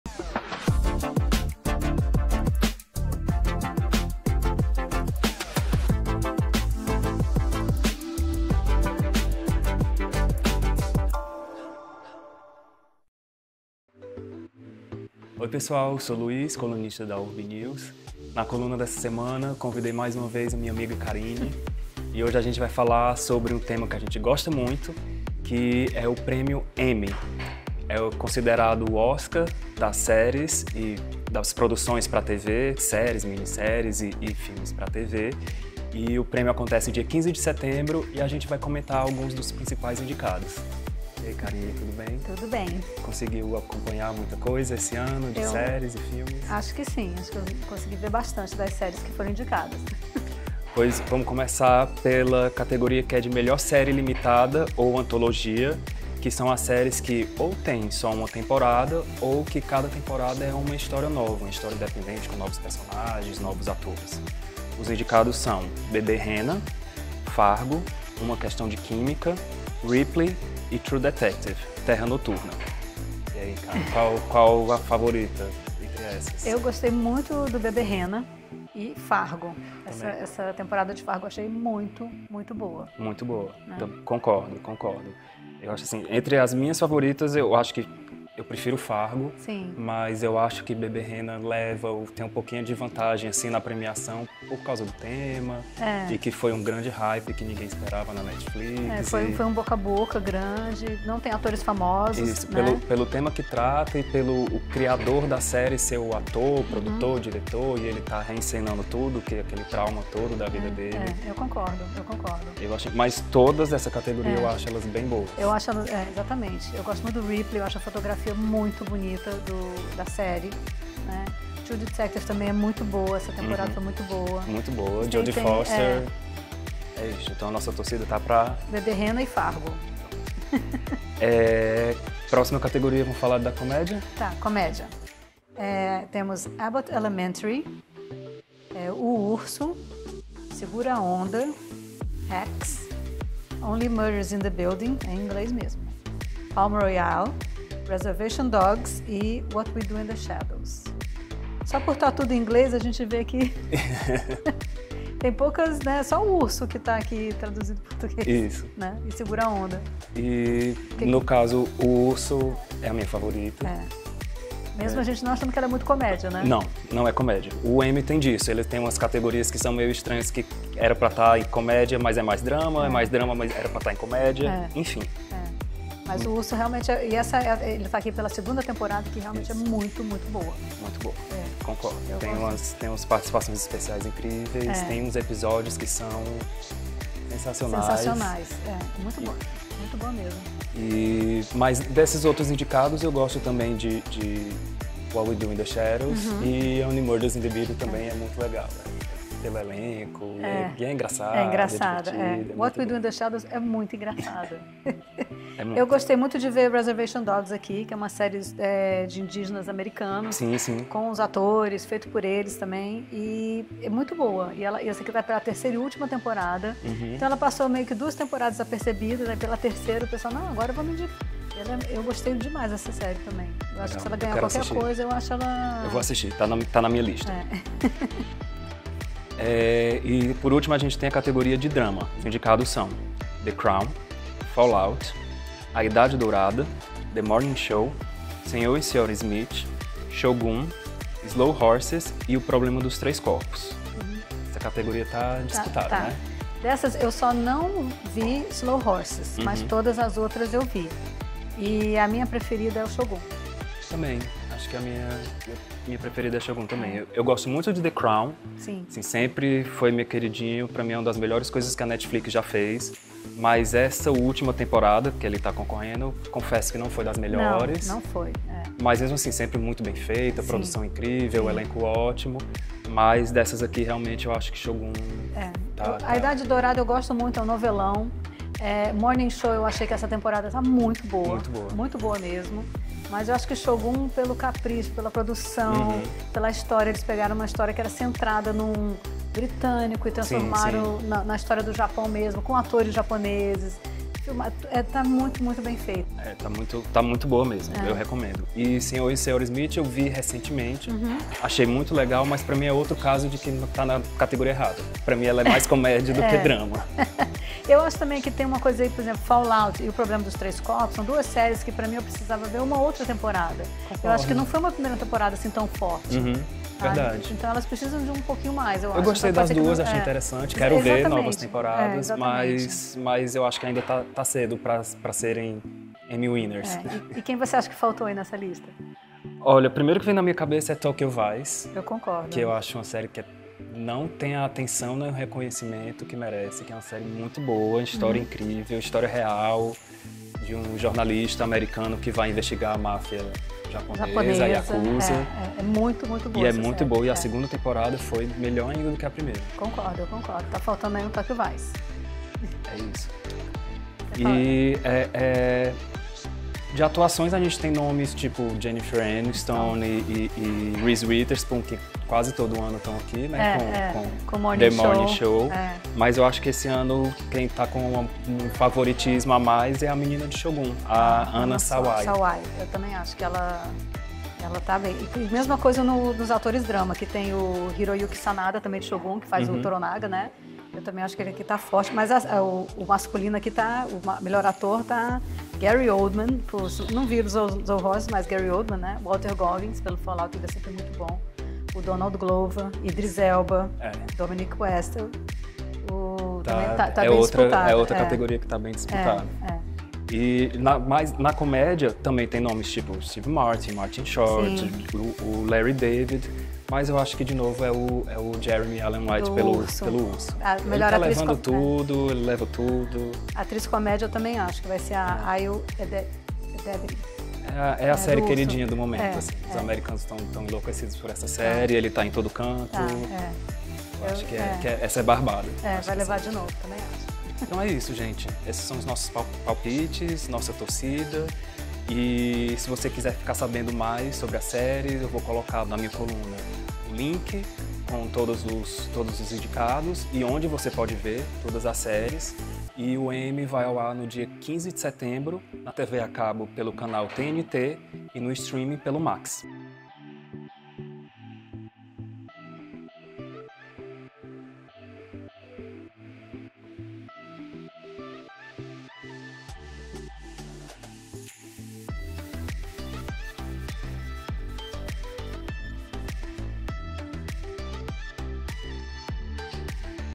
Pipoca&Arte: conheça os filmes e séries favoritos ao Prêmio Emmy 2024
Nosso influencer de cultura, Luiz Esteves (@luiz_esteves), traz na coluna Pipoca&Arte desta semana a amiga e cinéfila Karine Marseille para comentar sobre os principais favoritos ao Prêmio Emmy 2024.